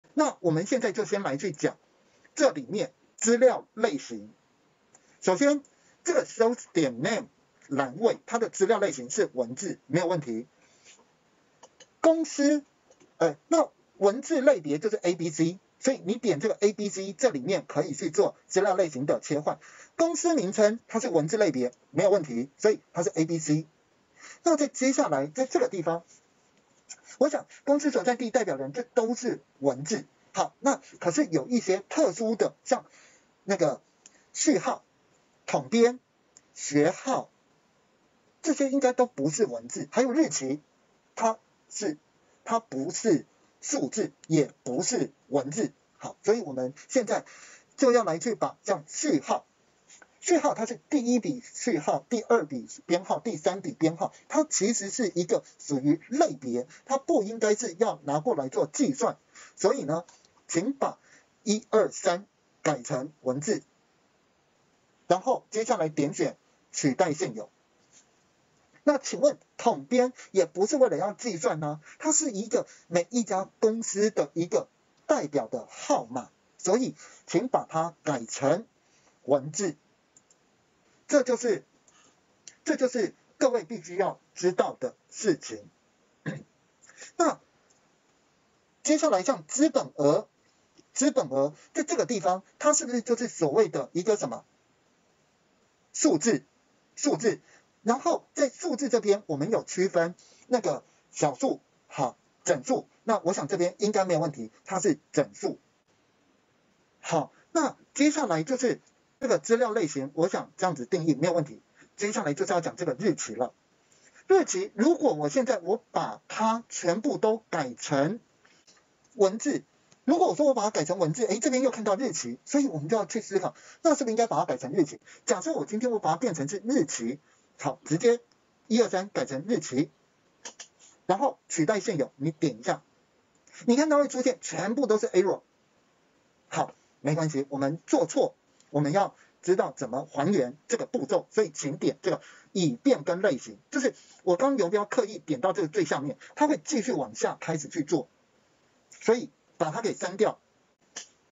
改變載入後欄位資料類型【EXCEL資料清理實務運用】07
【課程大網】

如何做資料類型的資料清理、如何做日期的資料清理、一個重新整理按紐完成新增資料也同時做完上述資料清理_Excel 資料清理